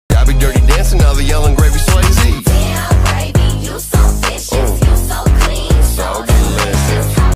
This is my favorite crossplay look 😍
Halloween 🎃  Is my favorite season for a make up and outfits outfits!  Get ready because I am so excited recreate fun looks this year😁!